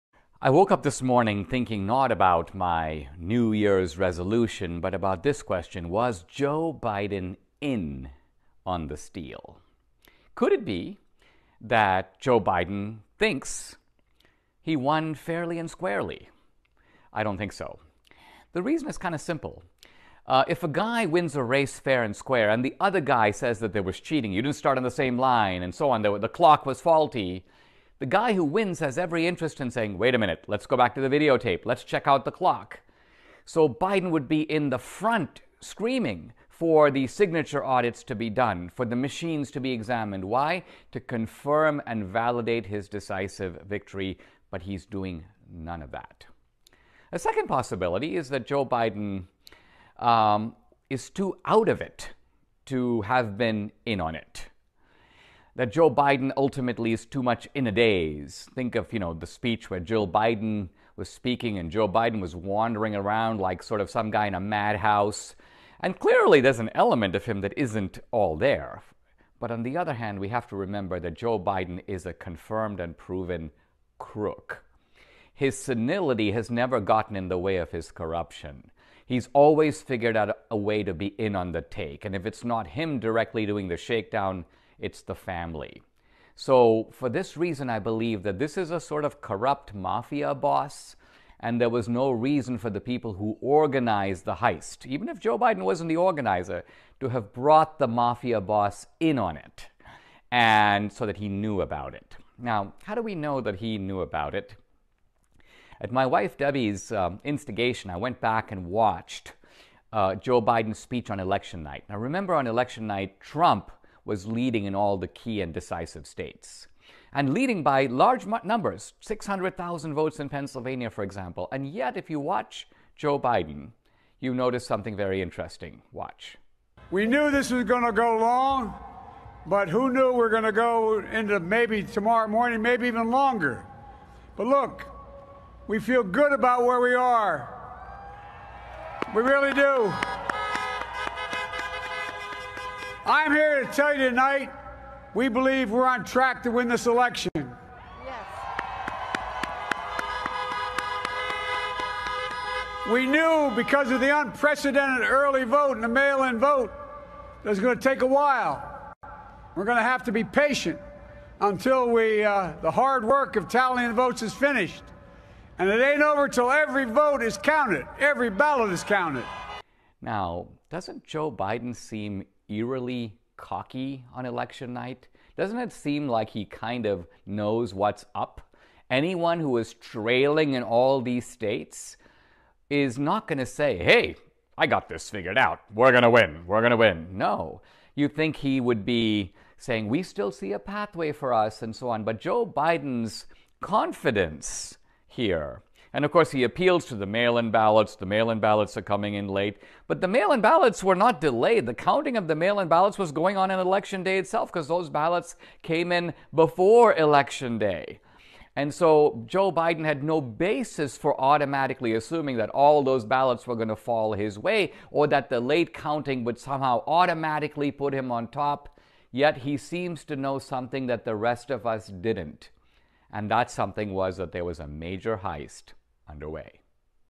Was Joe Biden in on the election fraud?
The Trump card author Dinesh D’ Souza talks about whether or not Joe Biden knew about all of the election fraud that occurred.